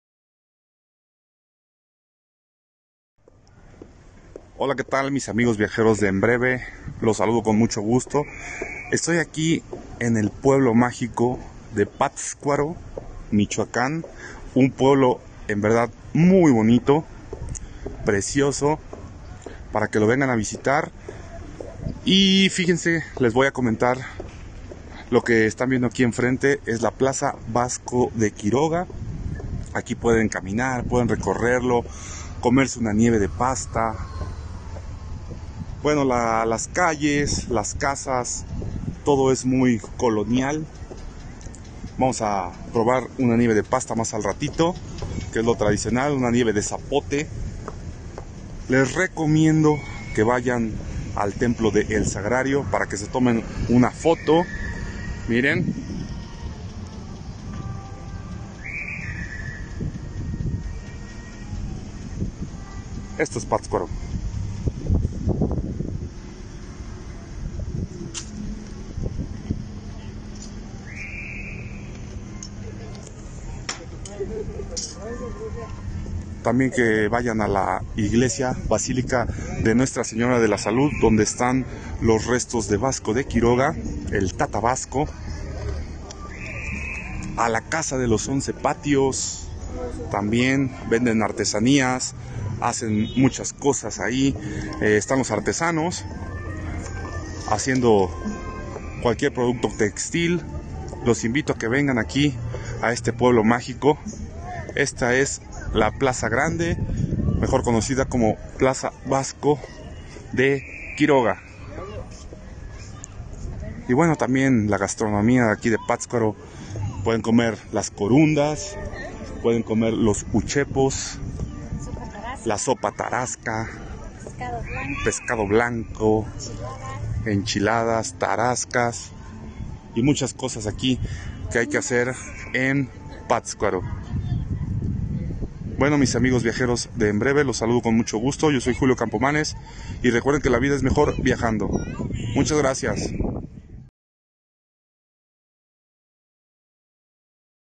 De visita en Pátzcuaro
Julio Campomanes nos invita a visitar este Pueblo Mágico enclavado en el estado de Michoacán.
EnBreve es un medio de comunicación en línea, con información social y cultural de Xalapa, artículos de interés general y cartelera de eventos.